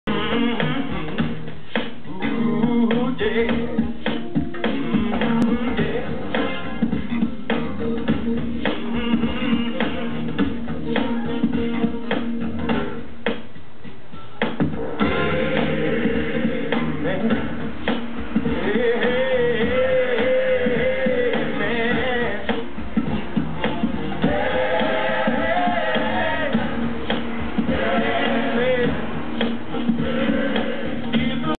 Norah funky dance
Norah danse chez Sophie, Olivier et Cristal, Nice juillet 2008